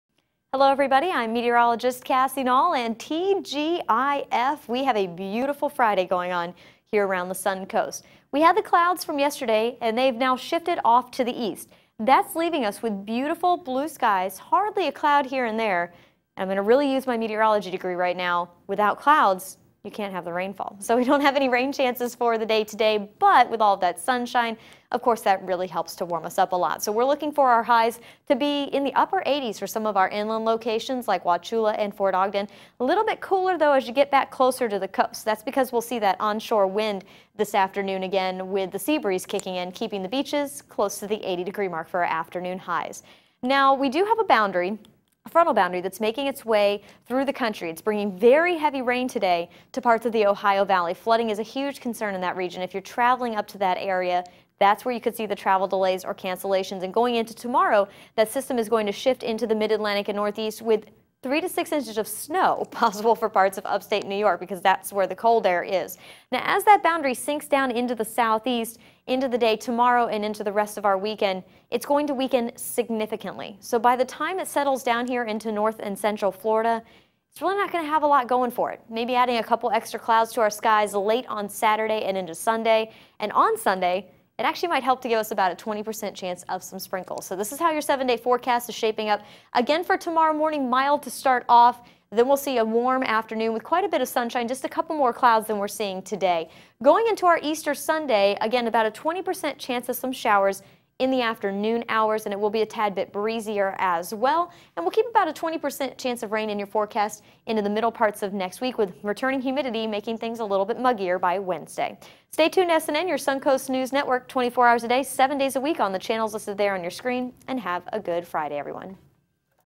SNN: Friday Weather Forecast 4/3/15
Plenty of sunshine today with warm temperatures and light winds will make for a very pleasant Friday on the Suncoast! Warm weather continues into the weekend with a slight chance of showers on Sunday afternoon as a weak front settles into the state.